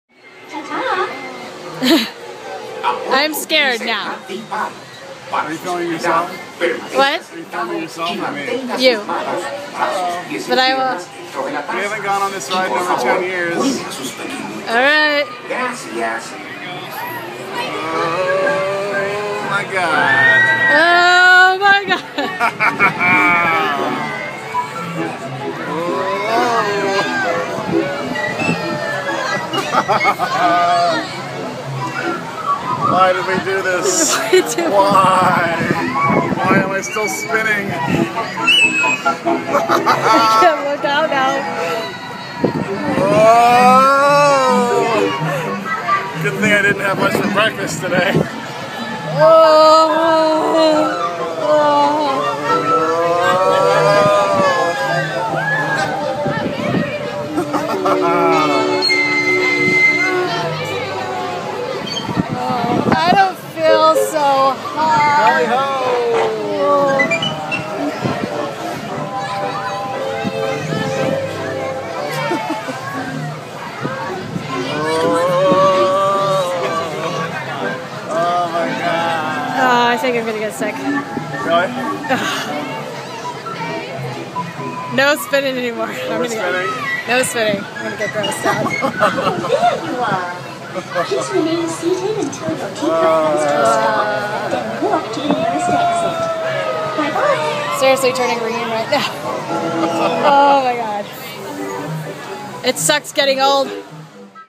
Mad Tea Party Teacups
Brad and I on the Teacups. We haven't been on it in years and now I know why. I get more dizzy now then when I was a kid.